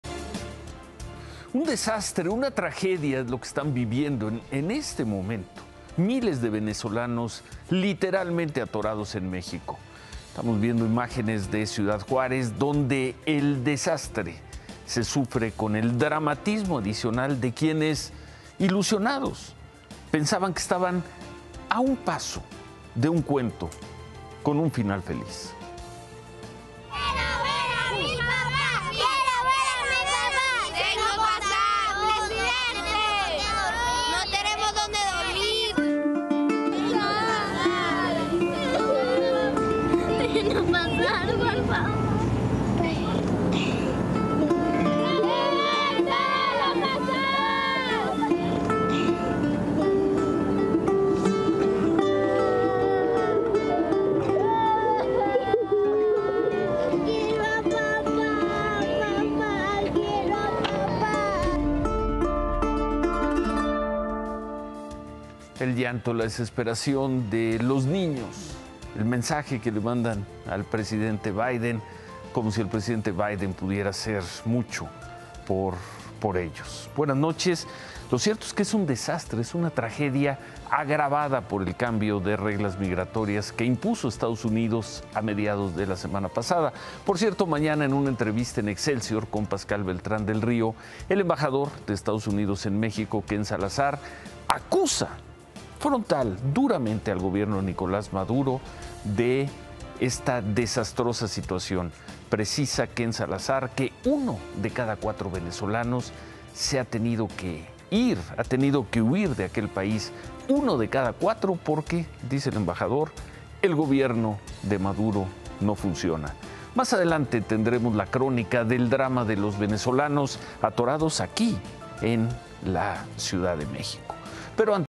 Miles de venezolanos viven una tragedia en México | Noticias con Ciro Gómez Leyva
CiroEnImagen “No tenemos dónde dormir”, dicen estas niñas venezolanas que forman parte de los miles de migrantes que están varados en México, tras la nueva política migratoria estadunidense. El embajador de EU en México, Ken Salazar, acusa al gobierno de Nicolás Maduro de esta desastrosa situación